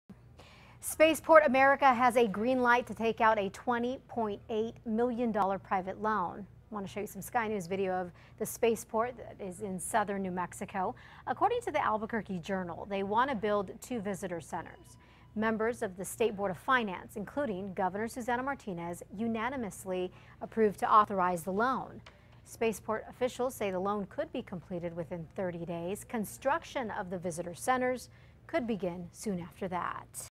Spaceport asks loan for visitor centers
Spaceport America, which was built with nearly quarter of a billion dollars in taxpayer money, is taking out a $21 million loan to build two visitor centers in southern New Mexico.